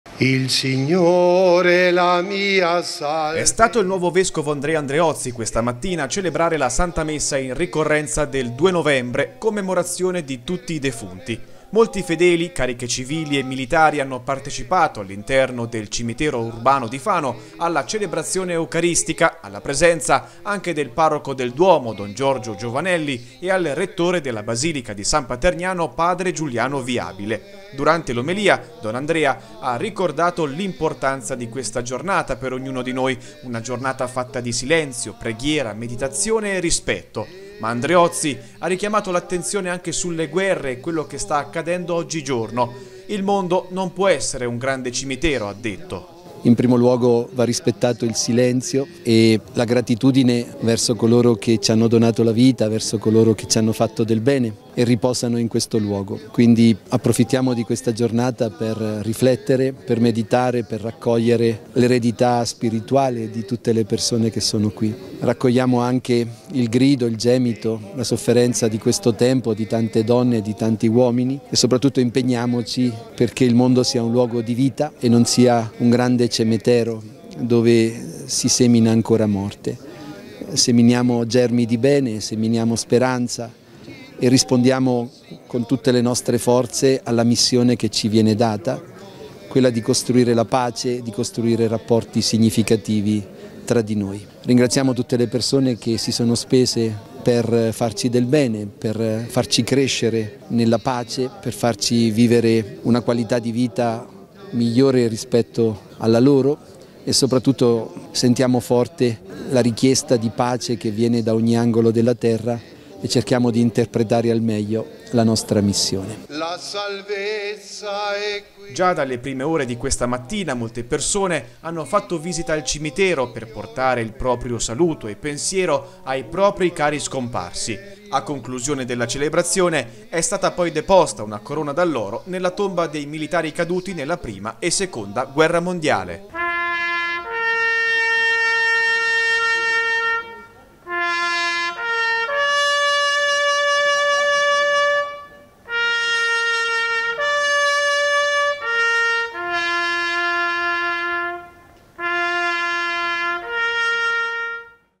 2 Novembre: a Fano la commemorazione dei defunti
In occasione della ricorrenza della commemorazione dei defunti, ieri mattina al Cimitero centrale di Fano è stata depositata una corona d’alloro al Cippo dei Caduti in guerra ed è stata celebrata una Santa Messa, presieduta dal nuovo Vescovo Andreozzi.